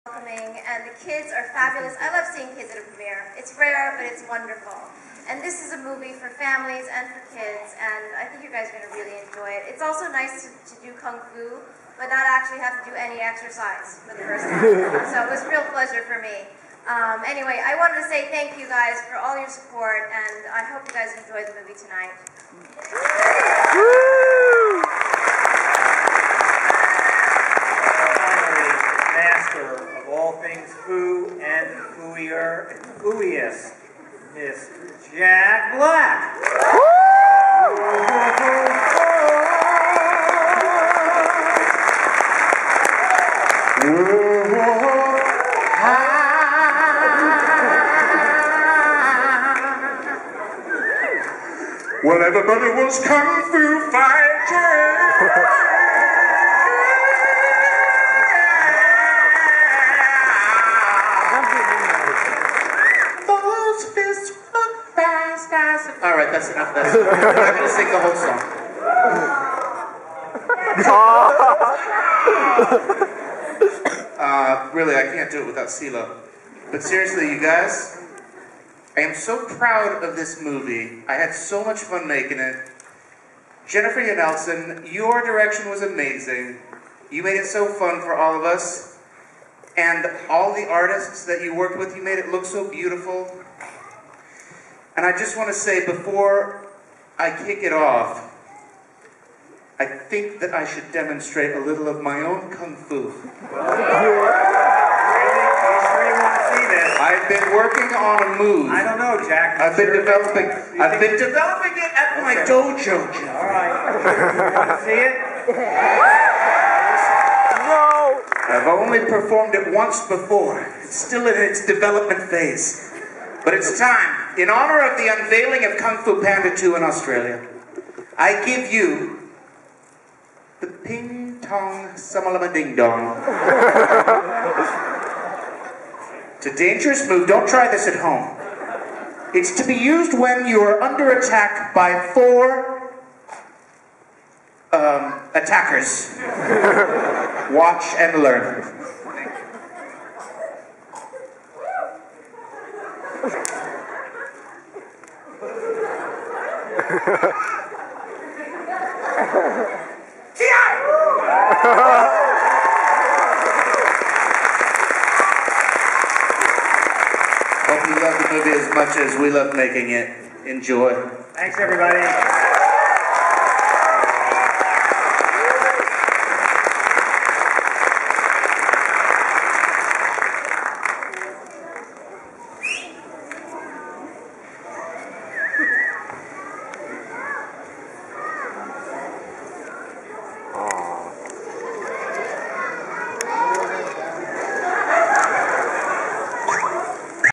Kung Fu Panda 2 Premiere - Pre-Screening Guests Presentation
On the 13th of June, 2010, TDIT attended the Kung Fu Panda 2 Australian Gala Premiere at Event Cinemas, George Street, Sydney. The star-studded event was graced by the actors Jack Black and Lucy Liu, director Jennifer Yuh Nelson and Dreamworks CEO Jeffrey Katzenberg.